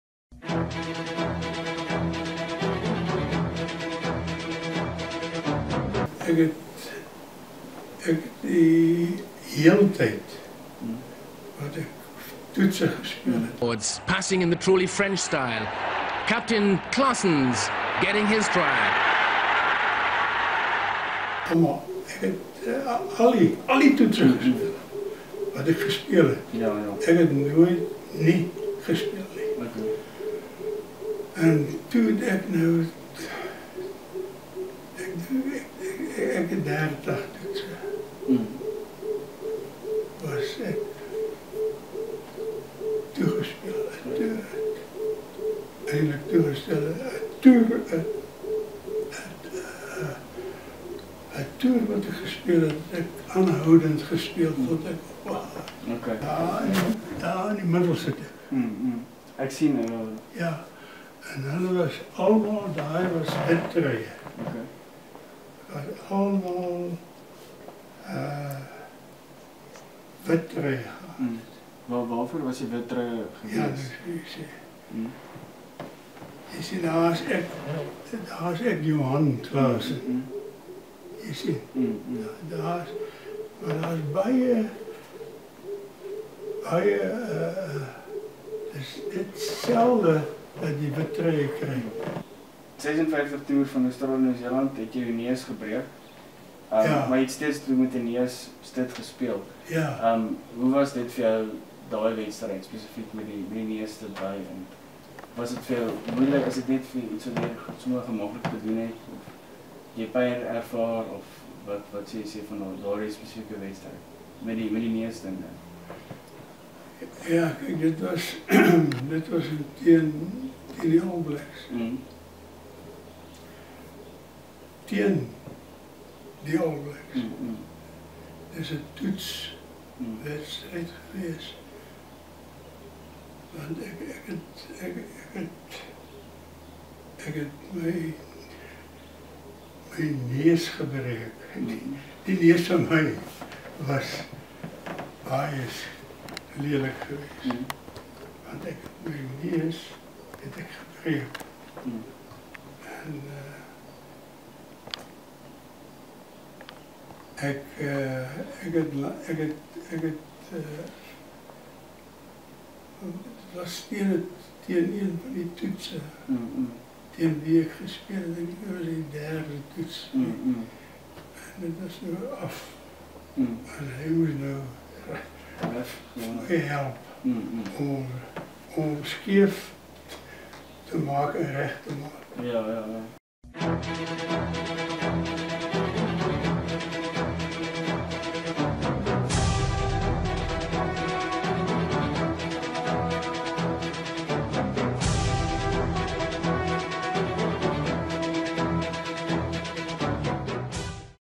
Herald gesels met Springbok-rugbylegende Prof. Johan Claassen
Potchefstroom Herald het met die oudste lewende Springbok-kaptein, Johan Claassen gaan gesels oor sy lewenslange betrokkenheid by rugby. Hier het Claassen 'n paar staaltjies vertel.